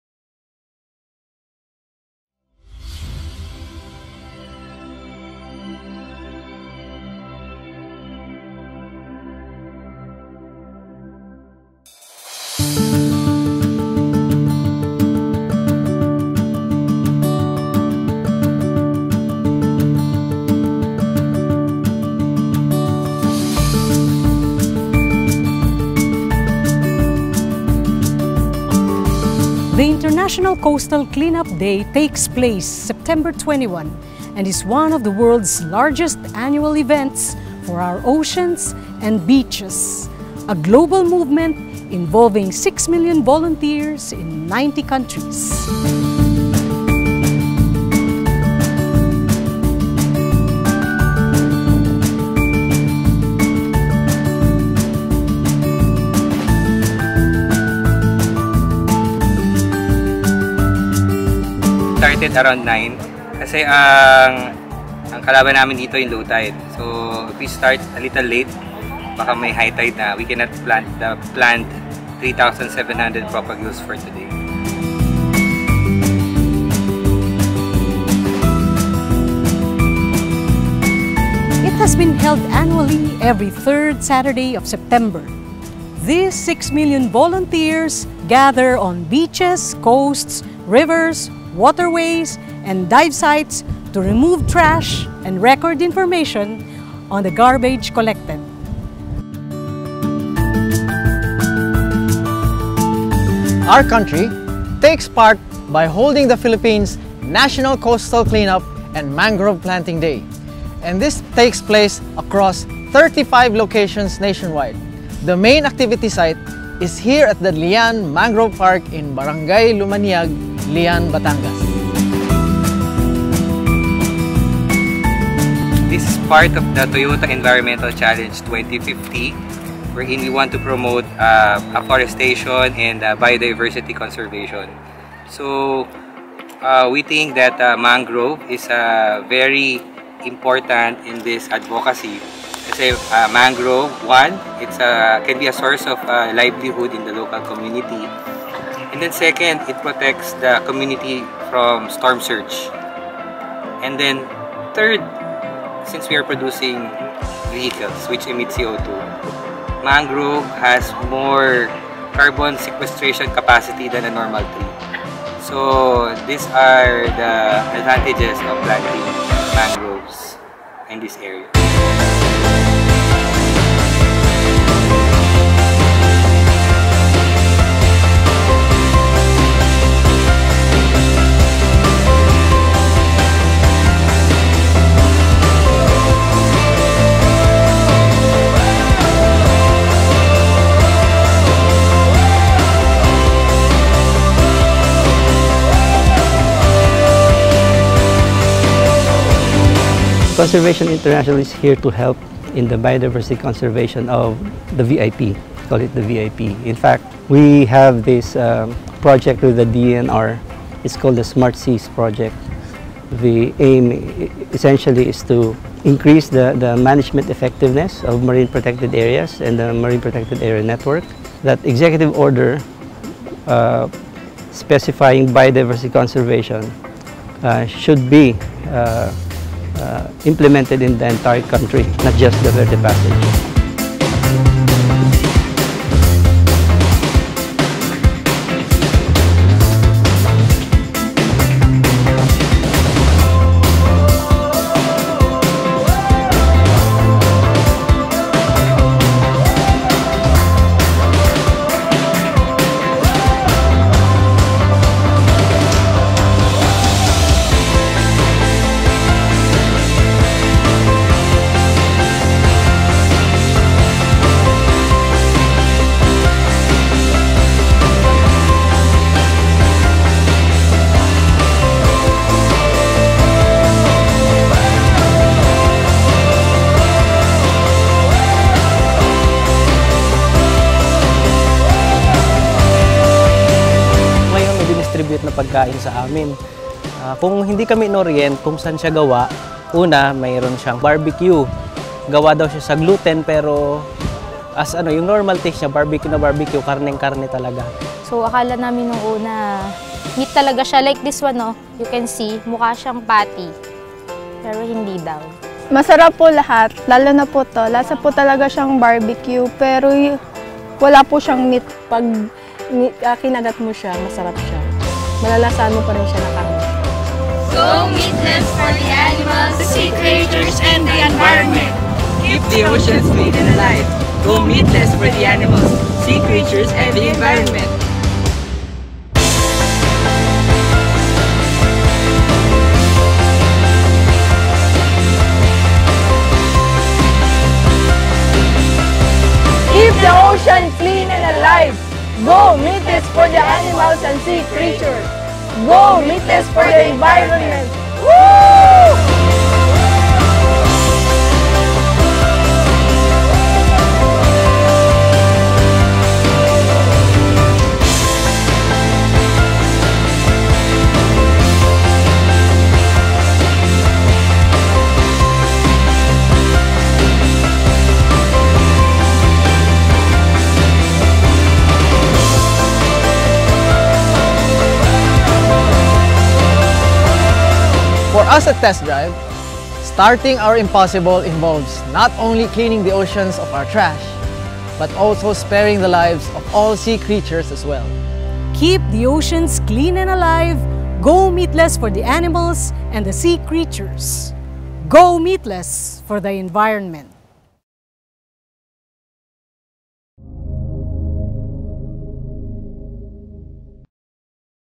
Paralympic athlete Ernie Gawilan joins coastal cleanup, mangrove planting with Toyota Motor PH
On September 21, 2019, TessDrive participated in the International Coastal Cleanup Day and Mangrove Planting upon the invitation of Toyota Motor Philippines (TMP). This was held at Barangay Lumaniag, Lian Batangas.

The coastal cleanup and mangrove planting activity also saw champion para-athlete Ernie Gawilan and fellow paralympic athlete Edwin Villanueva participate, along with the rest of the members of the Philippine Paralympic Committee.

Gawilan grew up and honed his swimming skills in a coastal community in Davao. He has been advocating for cleaner oceans.

Aligned with Toyota's global Start Your Impossible (SYI) campaign, Toyota Motor Philippines' initiative for the National Coastal Clean Up and Mangrove Planting Day is in support of its SYI campaign Hero Gawilan's advocacy for cleaner waters. Gawilan competed at the 2016 Summer Paralympics and was the first gold medalist for the Philippines in the Asian Para Games.

Gawilan also competed during the recently concluded Paris Paralympics 2024, finishing 6th in the swimming finals.